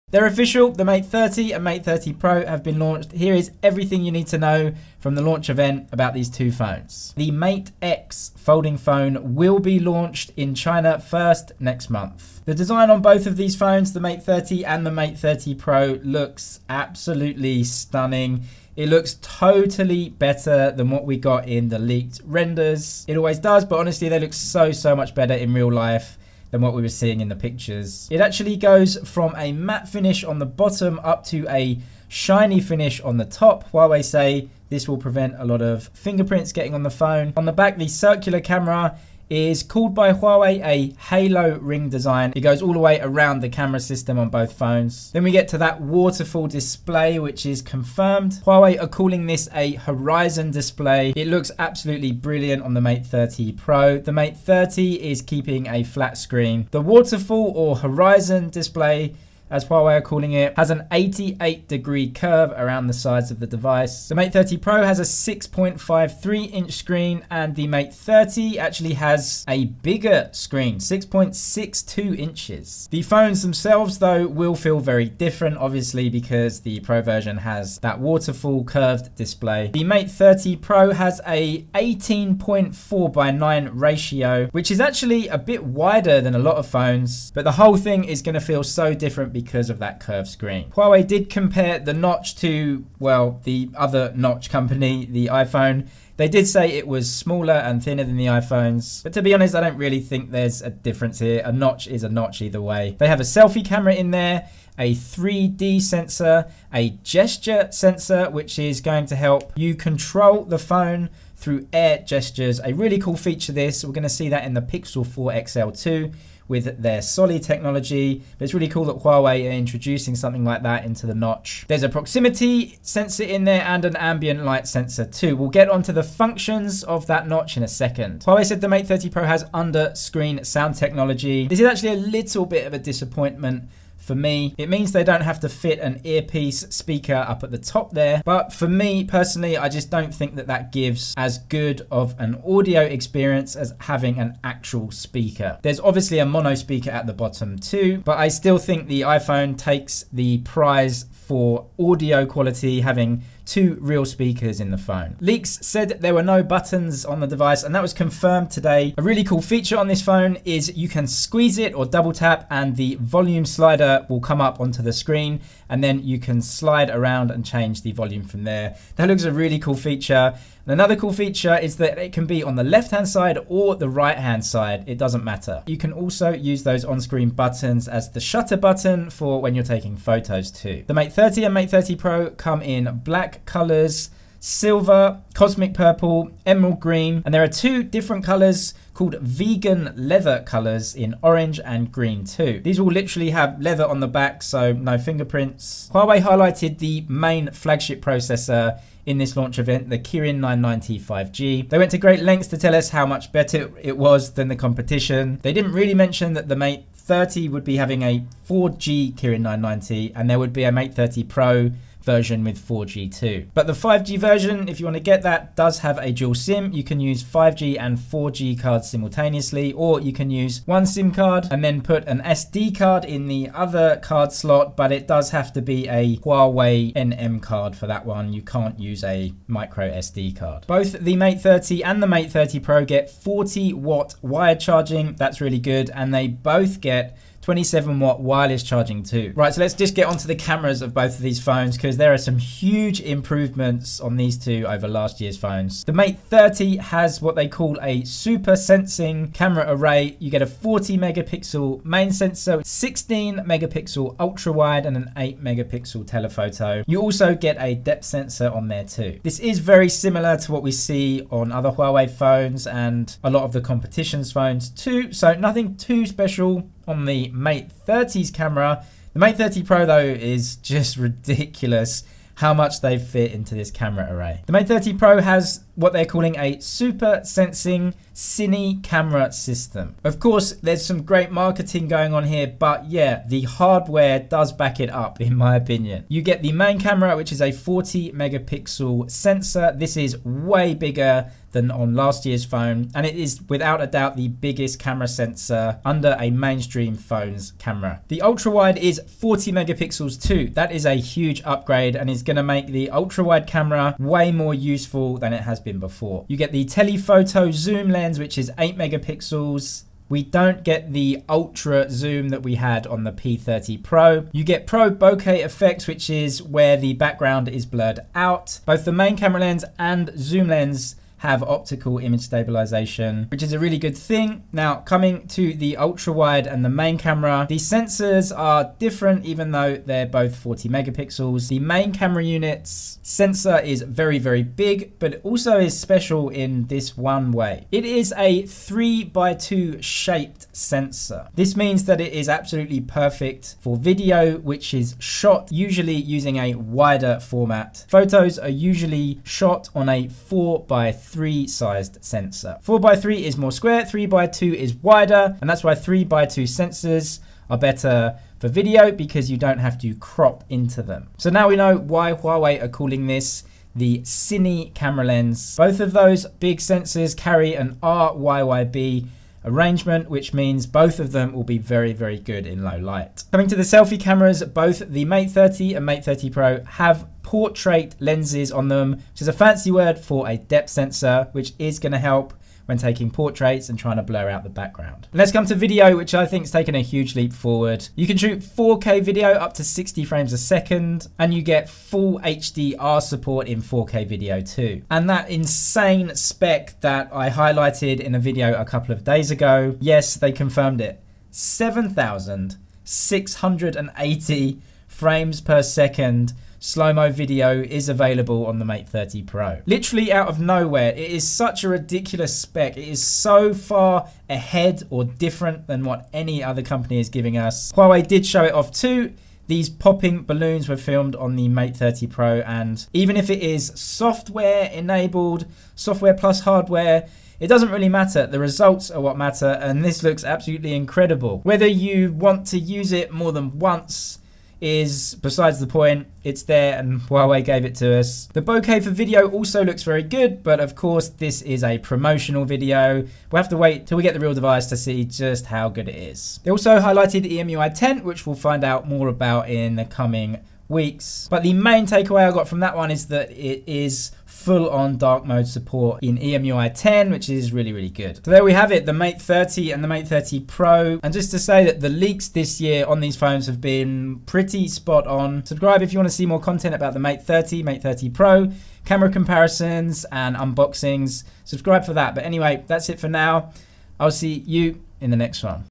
Huawei Mate 30 Pro Launched! Everything You Need To Know!
Huawei Mate 30 and Huawei Mate 30 Pro launched today with a host of upgrades for both models - here is the full run down of the launch event and hardware upgrades